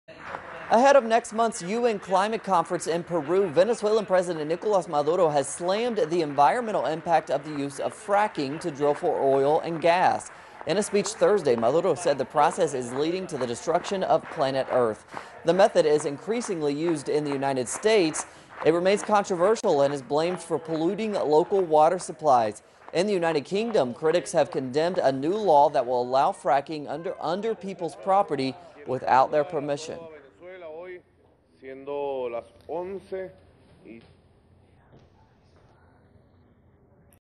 Venezuela's Maduro slams 'fracking'
Ahead of next month's UN climate conference in Peru, Venezuelan leader Nicolas Maduro slammed the environmental impact of fracking to drill for oil and gas. teleSUR